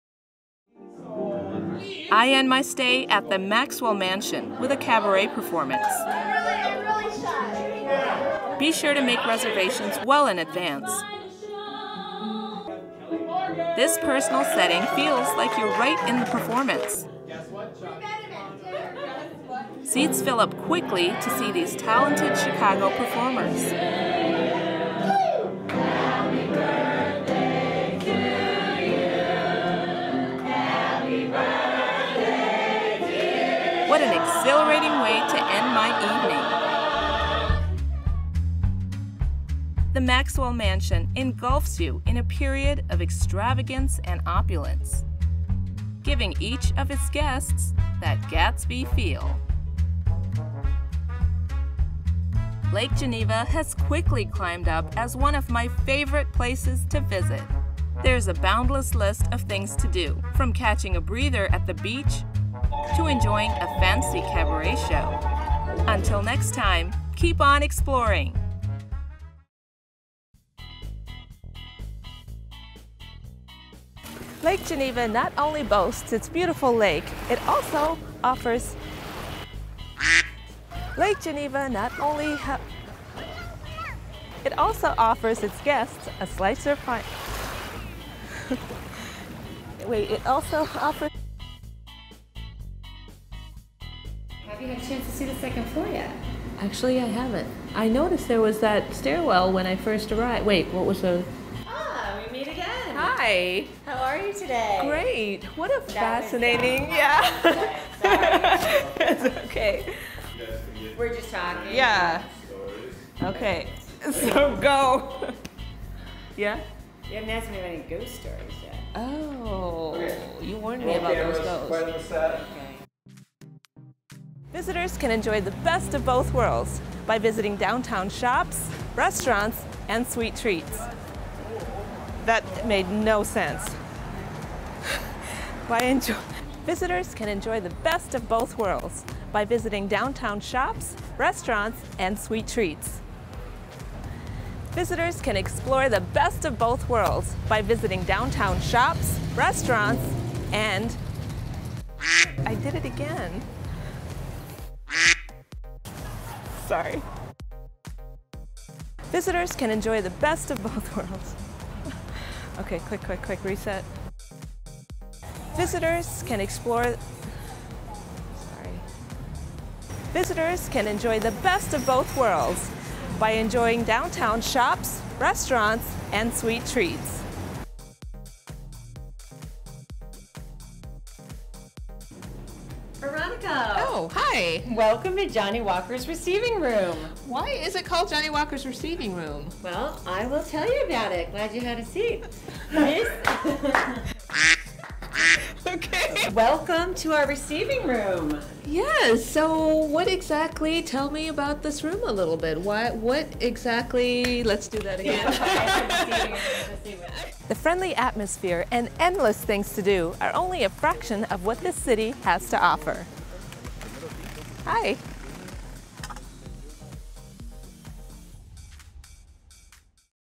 Lake Geneva Part 6 - Maxwell Mansion Cabaret & Bloopers
Wrapping up at the Maxwell Mansion, Veronica catches a Cabaret performance, a perfect way to end the stay in Lake Geneva!

In this episode, Veronica explores the popular tourist spot of Lake Geneva, Wisconsin. This breathtaking summer retreat offers various activities including the beach, parks & trails, shopping, water activities, and Mansion tours.